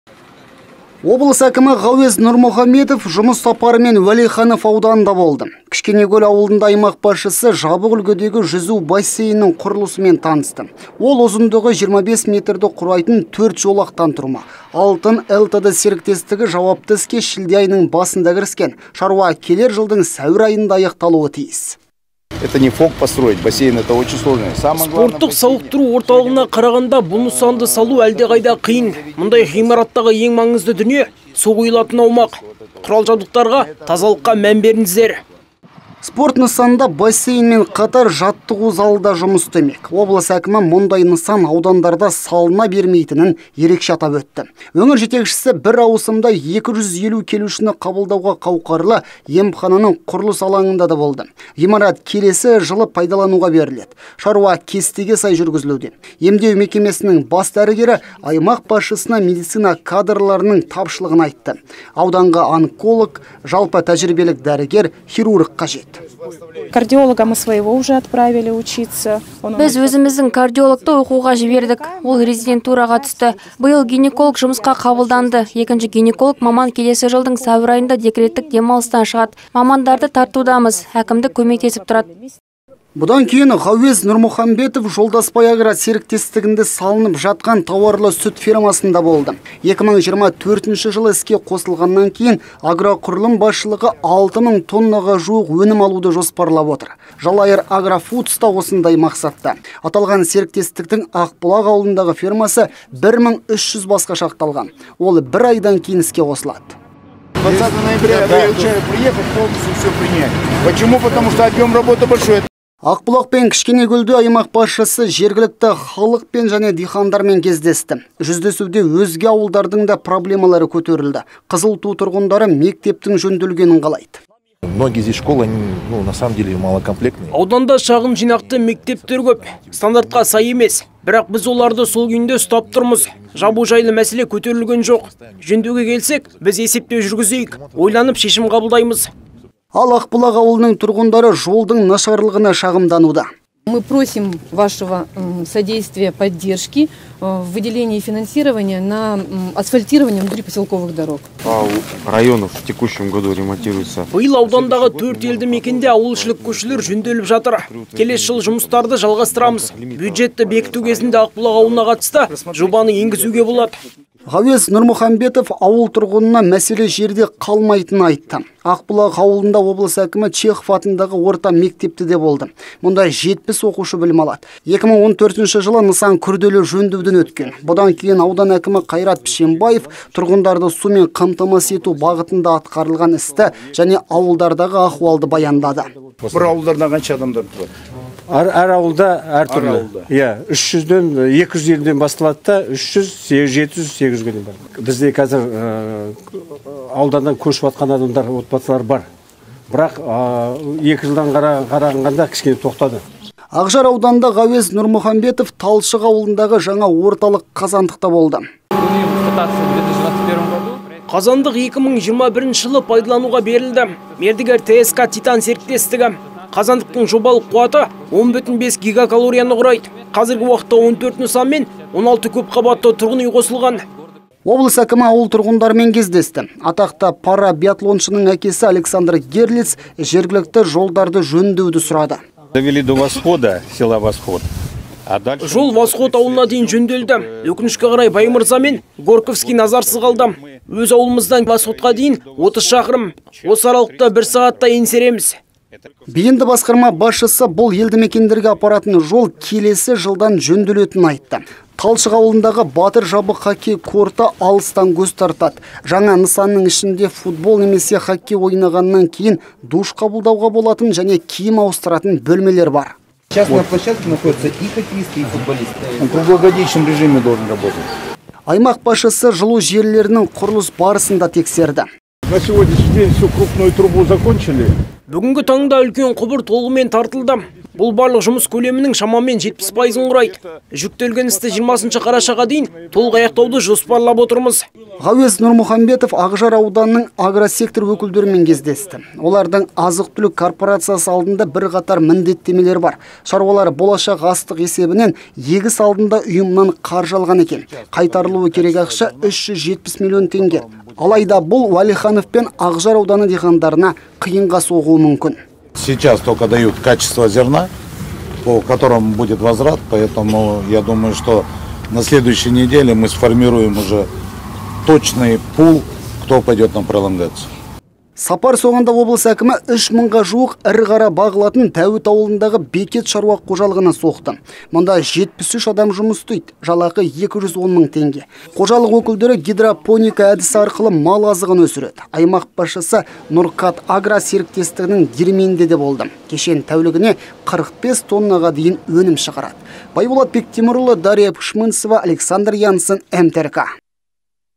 Ғауез Нұрмұхамбетов Уәлиханов пен Ақжар ауданында болды
Облыс әкімі Ғауез Нұрмұхамбетов жұмыс сапарымен Уәлиханов ауданында болды. Кішкенекөл ауылында аймақ басшысы жабық үлгідегі жүзу бассейнінің құрылысымен танысты. Ол ұзындығы 25 метрді құрайтын 4 жолақтан тұрмақ. «ALTYN LTD» серіктестігі жауапты іске шілде айының басында кіріскен. Шаруа келер жылдың сәуір айында аяқталуы тиіс.